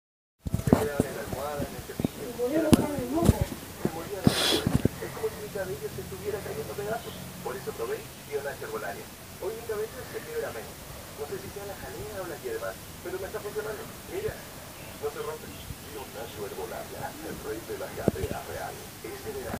TIO NACHO Herbolaria Milenaria 'Santiago Díaz' (2017)
Comercial de Tío Nacho Herbolaria: Santiago Díaz: "El cabello para mi es fundamental" "Gracias a Tío Nacho mi cabello no se rompe" . Tío Nacho Herbolaria. El Rey de la Jalea Real.